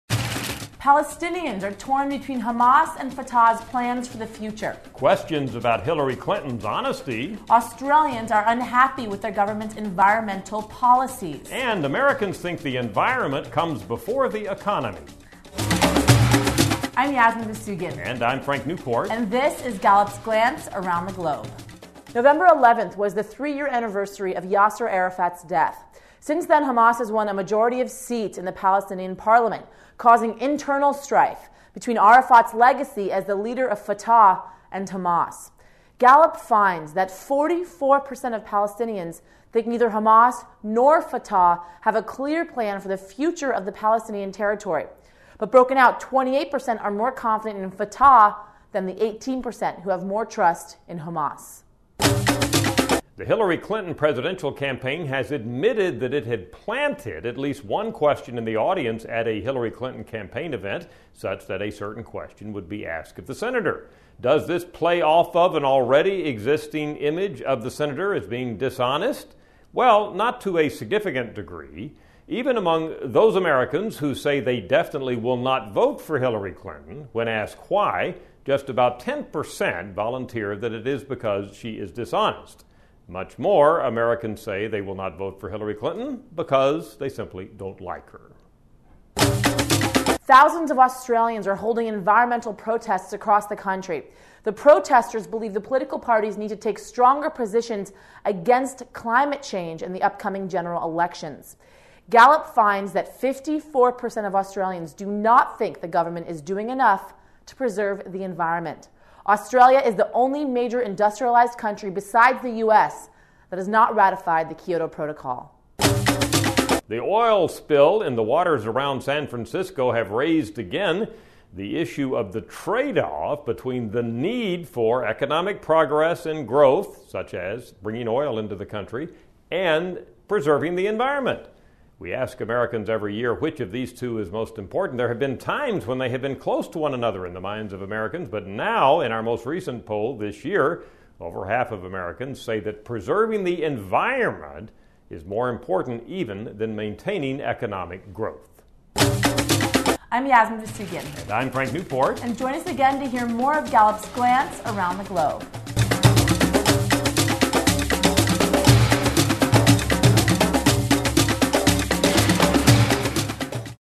Gallup Glance Around the Globe
Palestinians on Fatah and Hamas, Americans rate Hillary's honesty, Australians assess their government's environmental policies, and Americans prioritize the environment over the economy.